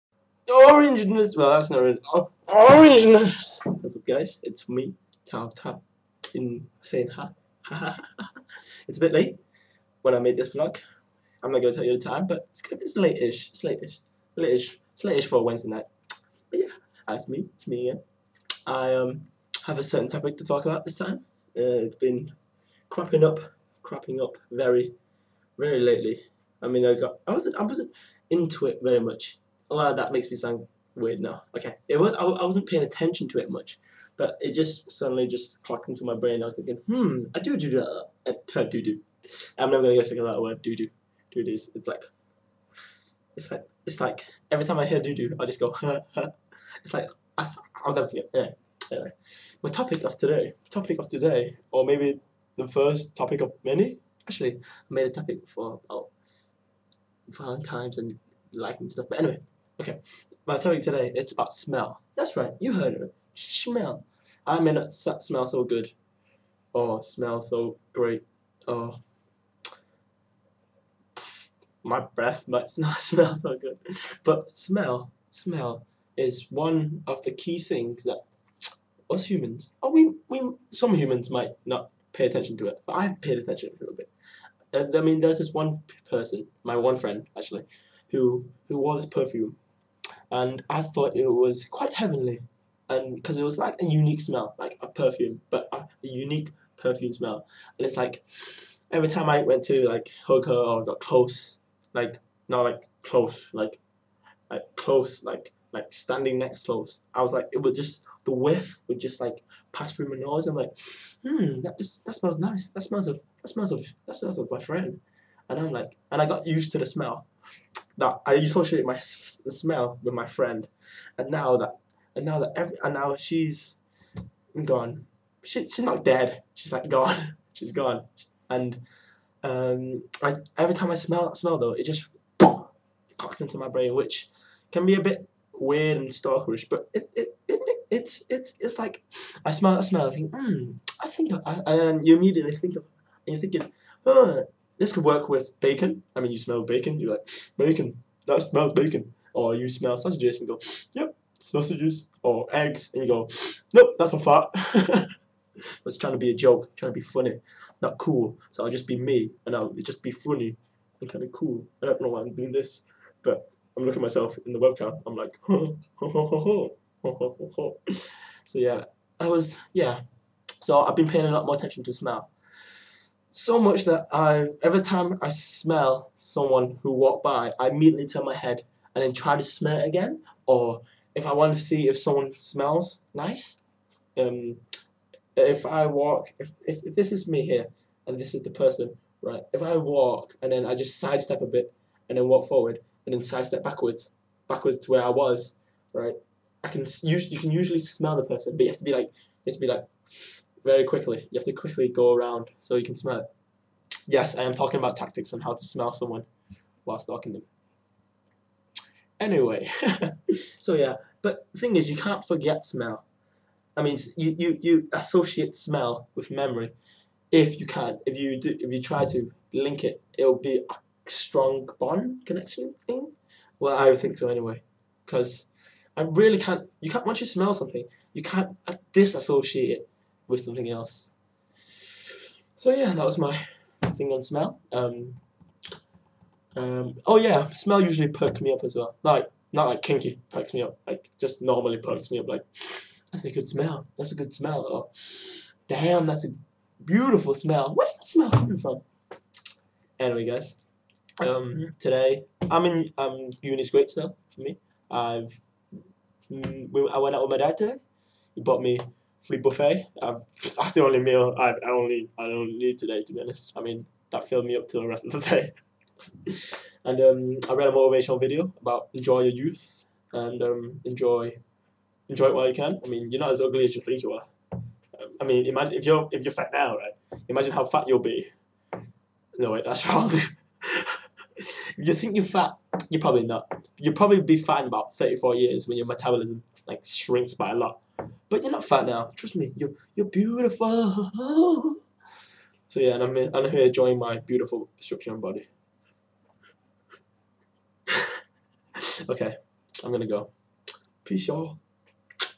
TOPIC: Smell (and my day today)
Today I will be going on about smelling shizz. Not literally though l0l
Also met up with my father today and step family, was good! He gave me money and I always feel guilty when I take money in cash from anyone :{{
In other news, I can now eat properly for a while yaaay! Still need to go to the doctors though.

Wear Suncreen!